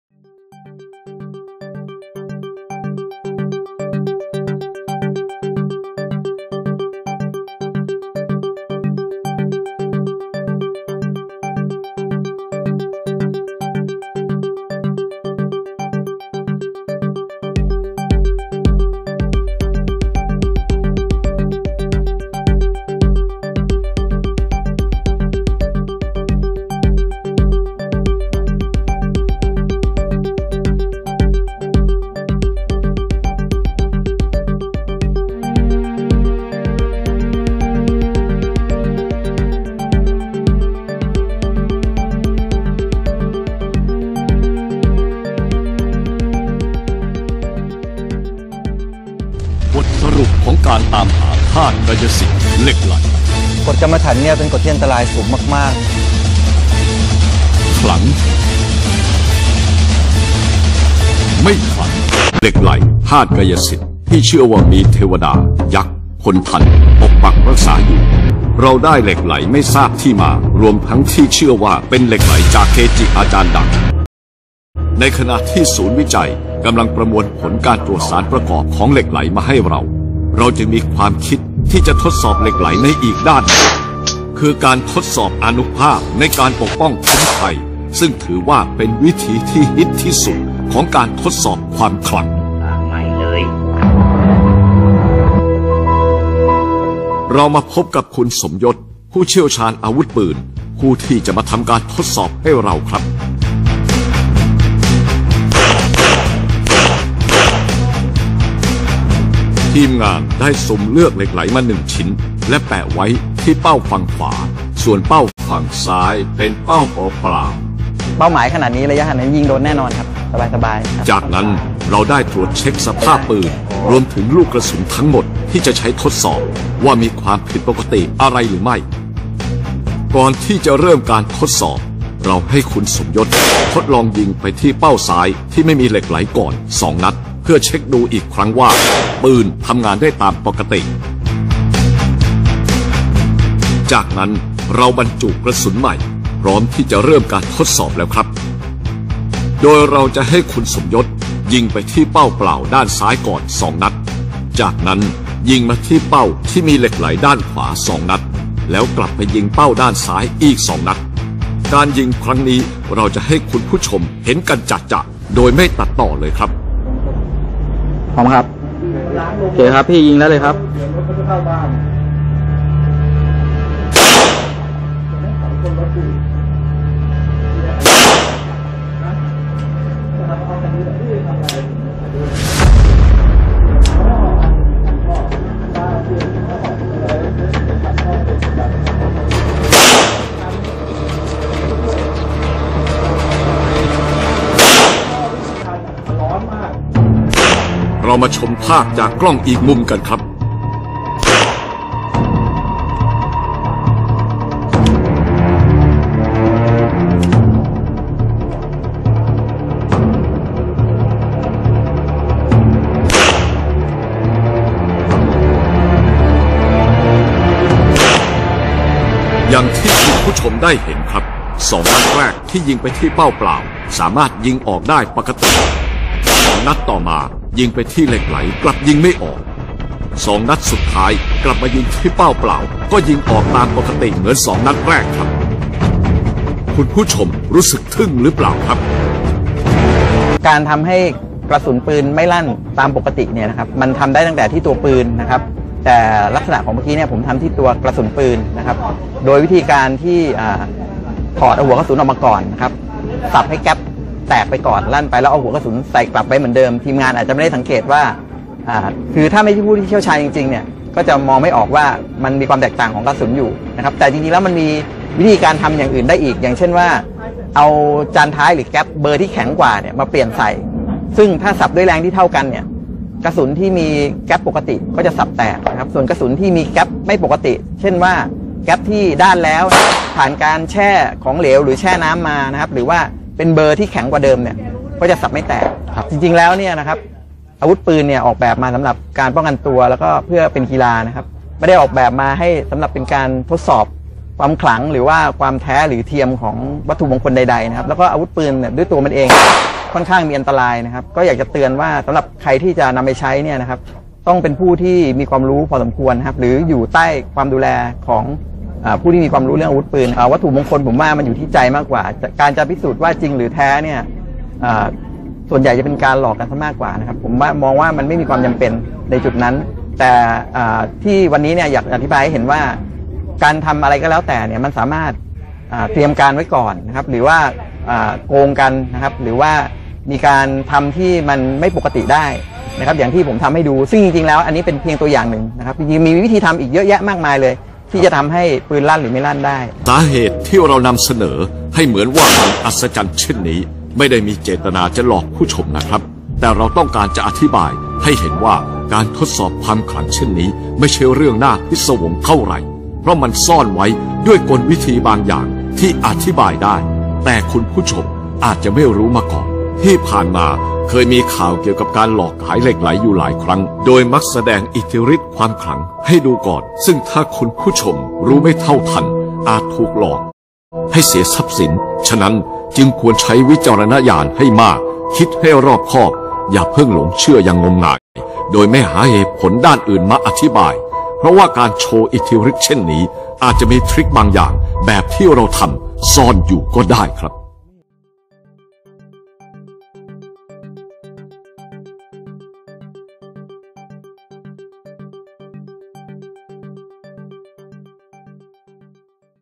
Power of Lek Lai Bulletproof Buddha Magic Documentary
Lek Lai Kaya Siddhi Elemental substance - a debunker documentary by Thai Channel 5. The documentary does not debunk anything successfully, because no real shots are ever fired at the Lek Lai, so nobody knows if it would have gone off